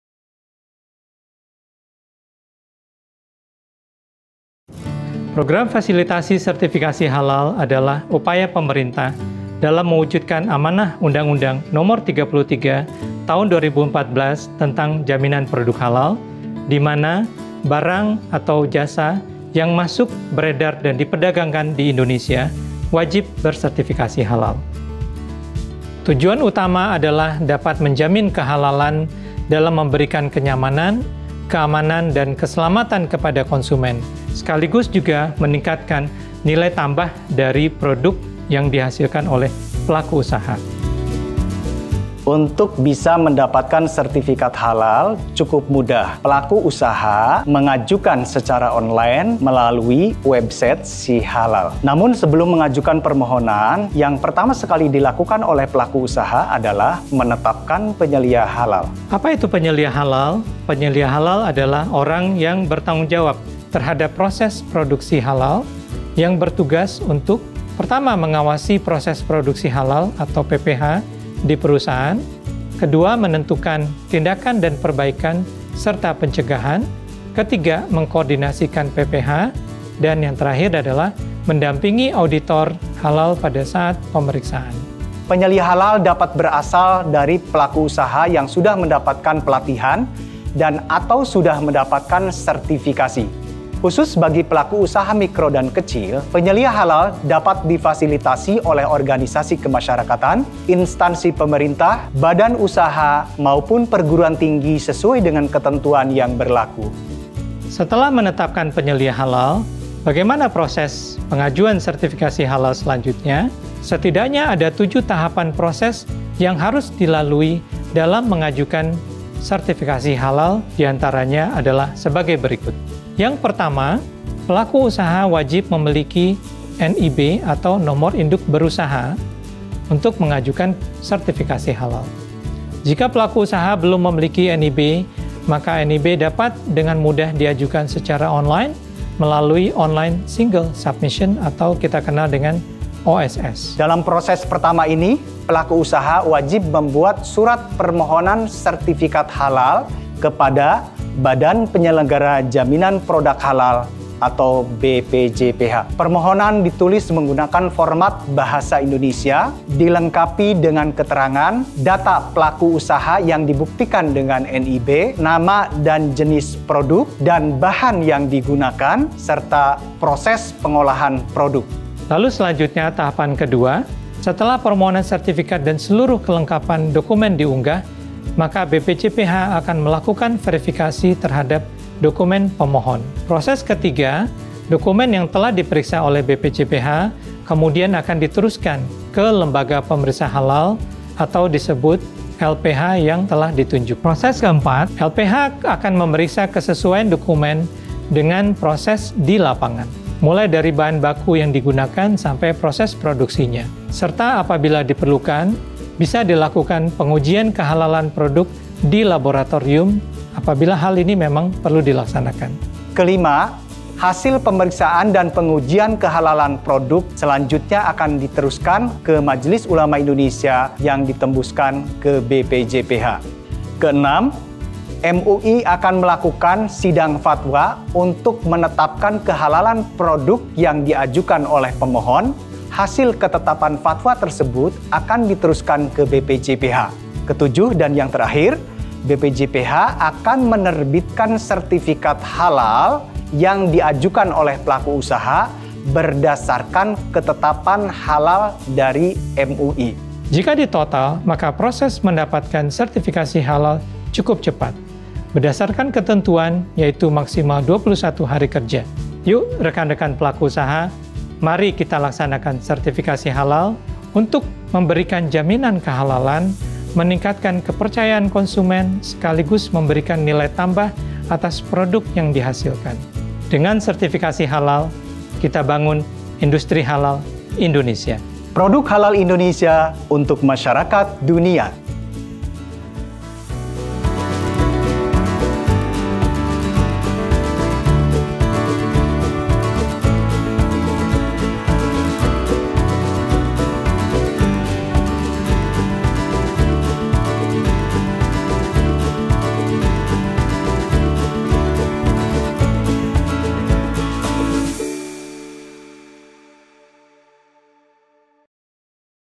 Testimoni Sertifikasi Halal
Dalam video ini, SobatEksyar akan diberikan penjelasan mengenai
- Pentingnya sertifikasi halal
- Apa itu Penyelia Halal
- Proses pembuatan sertfikasi halal
- Dampak memiliki sertifikasi halal

Yuk SobatEksyar dan rekan-rekan pelaku usaha mari kita laksanakan Serifikasi Halal, untuk memberikan Jaminan Kehalalan, Meningkatkan Kepercayaan Konsumen, dan juga memberikan Nilai tambah atas Produk yang dihasilkan.